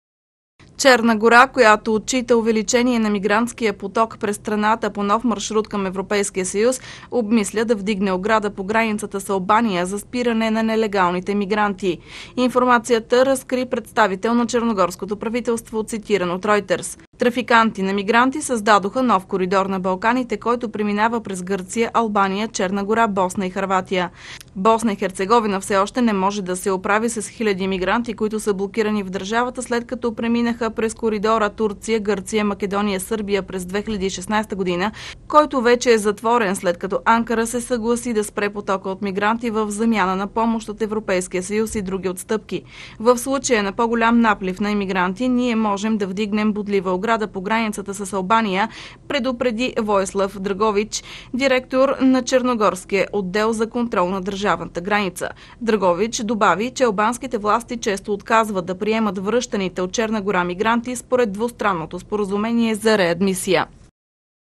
Мерки. Черна Гора може да вдигне ограда по границата с Албания заради мигрантите /21.05.2018 г./
Черна гора, която отчита увеличение на мигрантския поток през страната по нов маршрут към ЕС, обмисля да вдигне ограда по границата с Албания за спиране на нелегалните имигранти. Информацията разкри представител на черногорското правителство, цитиран от "Ройтерс"